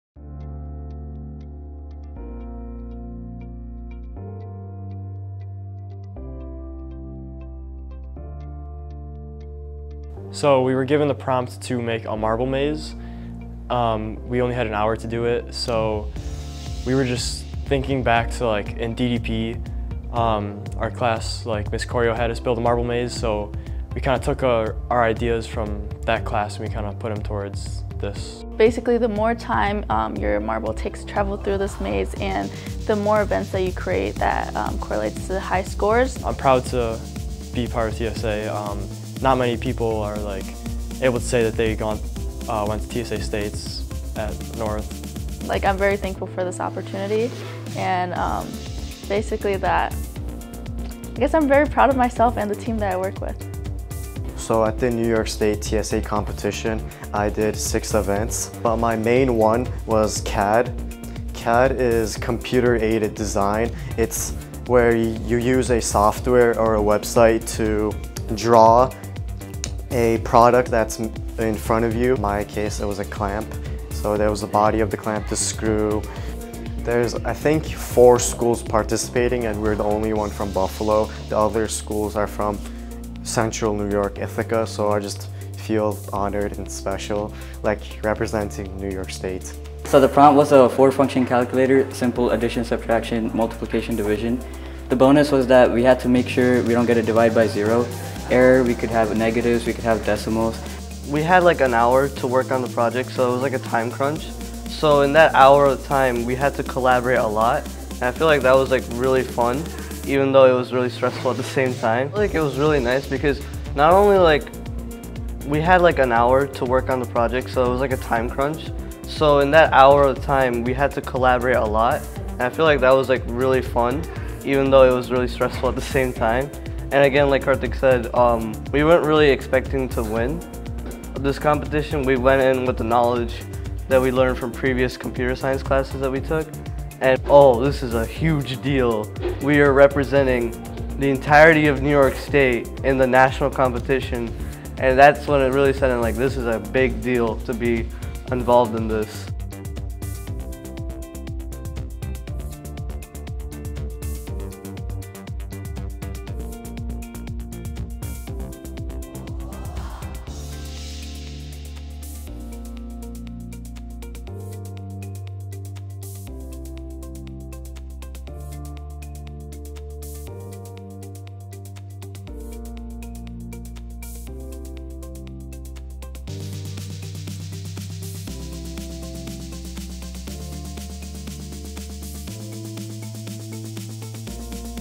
District Spotlight - Williamsville North Technology Student Association advances to Nationals
The following students earned a Top 3 finish and Qualified for the National Conference to be held in June.

Coding - 1st place, Karthik Bhagavatula & Pranav Vaddadi
Technology Problem Solving - 2nd place, Josh Anand, & Grant Pilozzi
Audio Podcasting - 2nd place, Mya Sliwinski
Coding - 3rd place, Josh Anand & Aarish Khan
Dragster Design - 3rd place, Ryan Garrestson
Music Production - 3rd place, Karthik Bhagavatula, Yavuz Gebes, Mohammed Shomik, Jay Singh, Brahmjot Singh, and Pranav Vaddadi
Photographic Technology - 3rd place, Yavuz Gebes
Technology Problem Solving - 3rd place, Connie Chen & Carter Miller

The following students have qualified to represent North's chapter events at the National Conference:

Grace Banks
Anton Danilovich
Alexei Goubski
Poornima Kandarpa
Eugene Kim
Michael Mommertz
Hadia Muzzammil
Tim Park
Peter Park
Nathan Romito
Leander Zhai